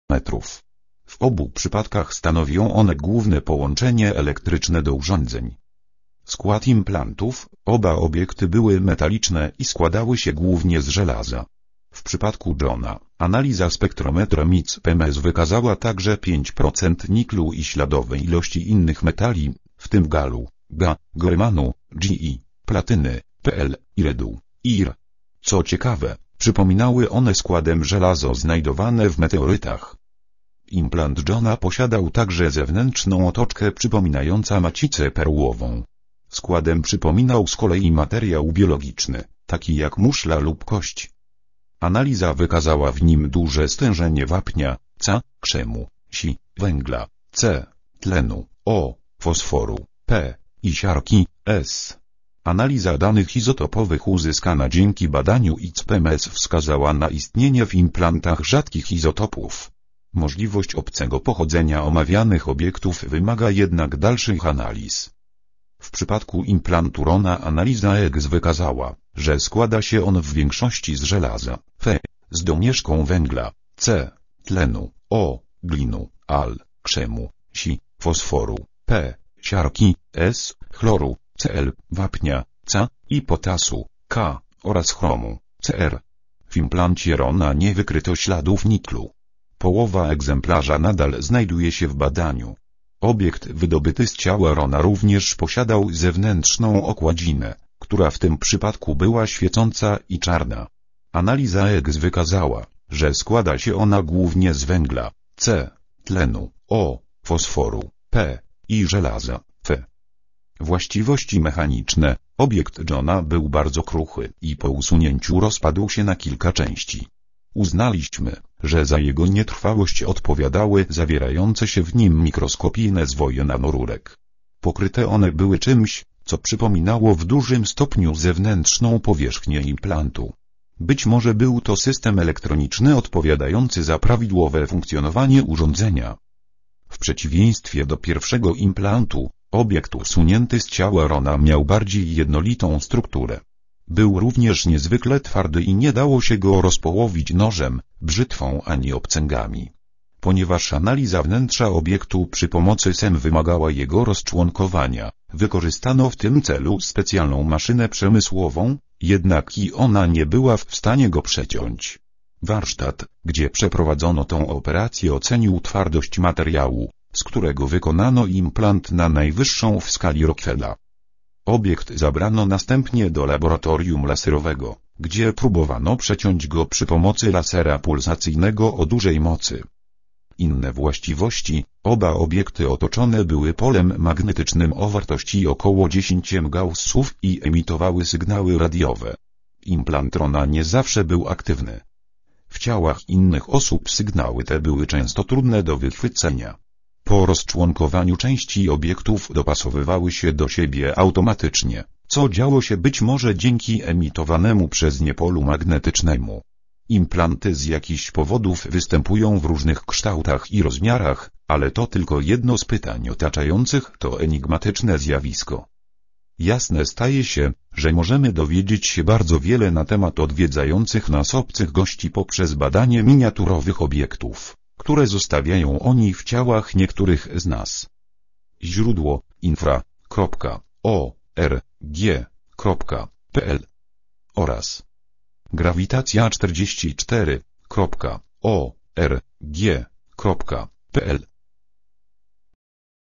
[2/2] Implanty-pozaziemska technologia monitoringowa
Implanty z jakichś powodów występują w różnych kształtach i rozmiarach, ale to tylko jedno z pytań otaczających to enigmatyczne zjawisko.